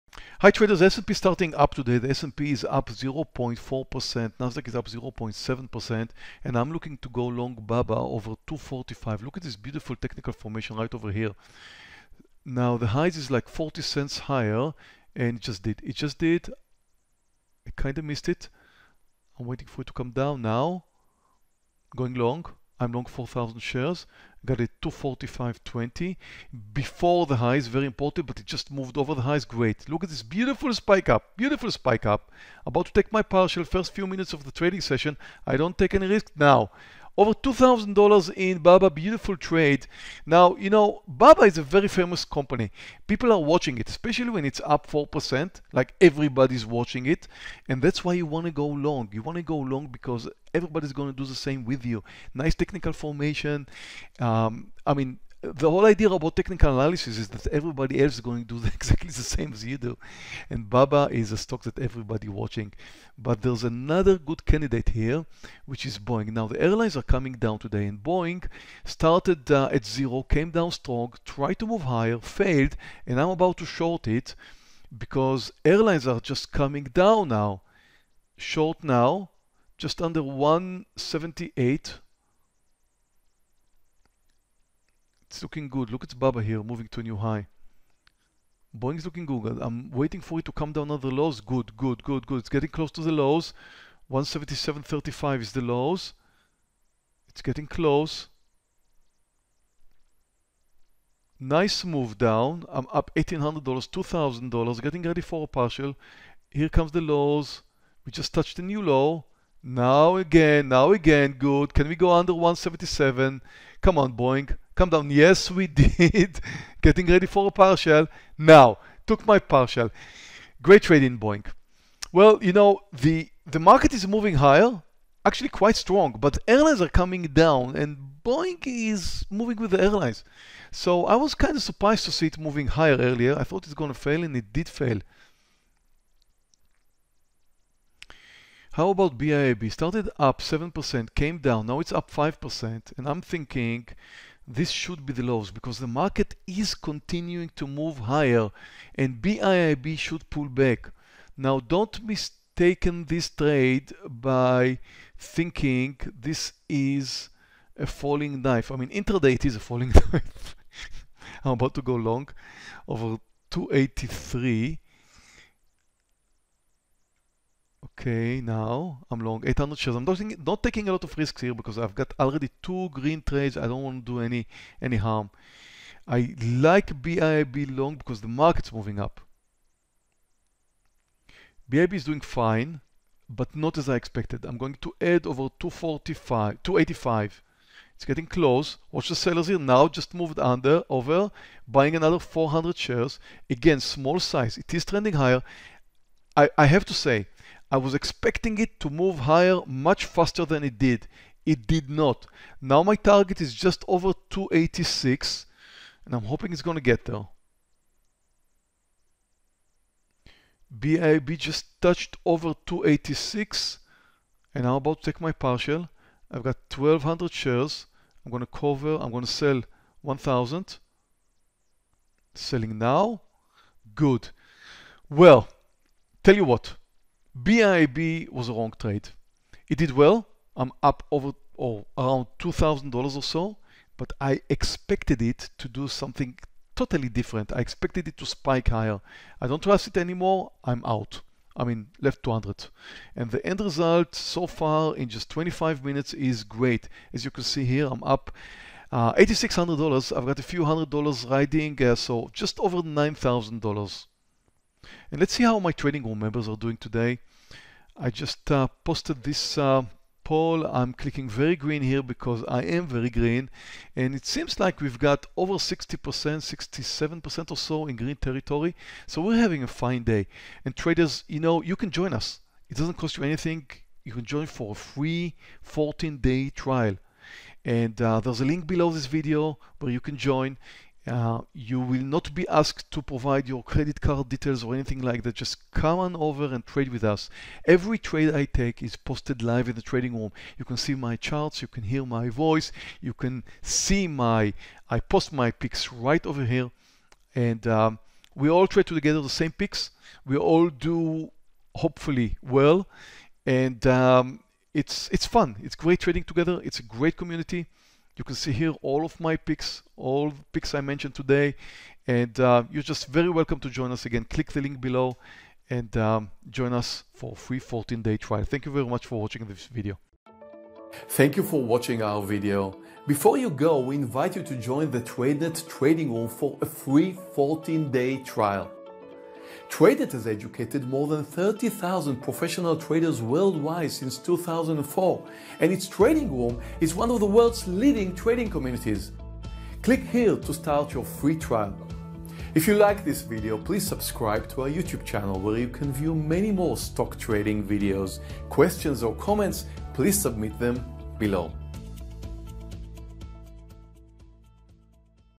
Live Day Trading for $9,000 in Profits
Live Day Trading Stocks - Earning  $9,000 day trading BA, BIIB and BABA. Meir Barak live day trading on July 9th. 2020.